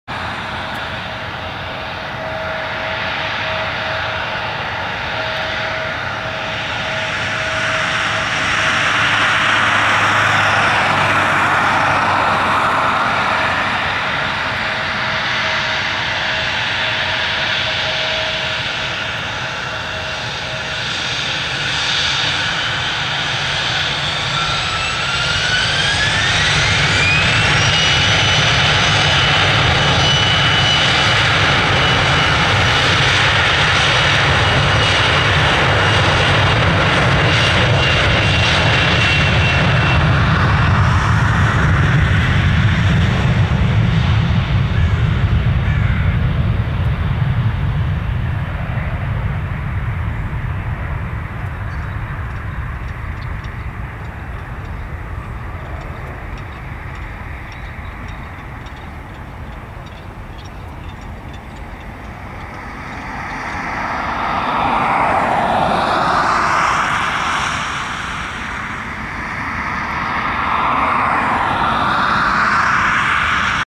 American Eagle (Compass Airlines) ERJ-175 [N206NN] Takeoff Portland Airport (PDX)
Flight: CPZ6025 PDX-LAX